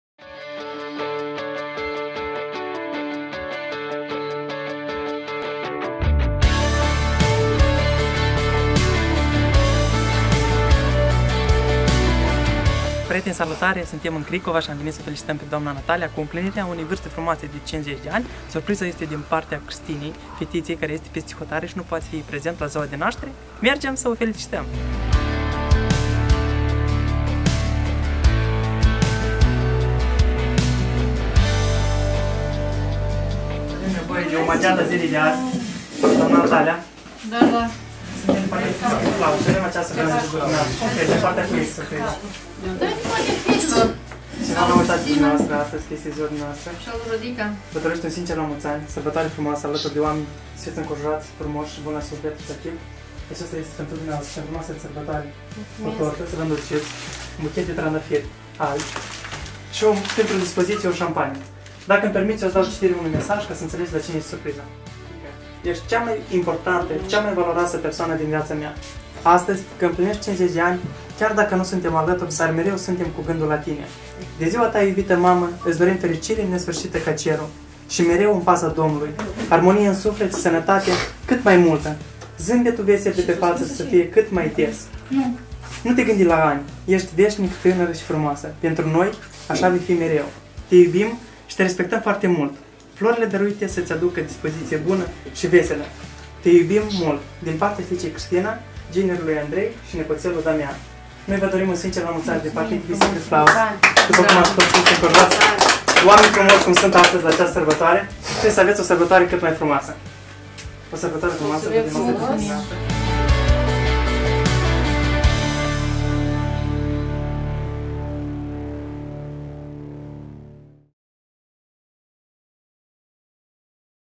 Livrare Secretflowers pentru Dna. Natalia.
Un gest poate readuce zimbetul pe fața părinților noștri.
Noi te ajutăm cu alegerea.
Intra pe alege cadoul dorit, iar noi il vom livra în cel mai scurt timp si cu emotii pozitive!